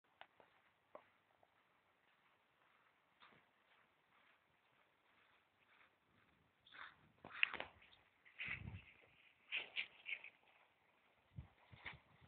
Rikke på høsttur
Rikke, pappa og Jenny på tur - høstlig med masse laug.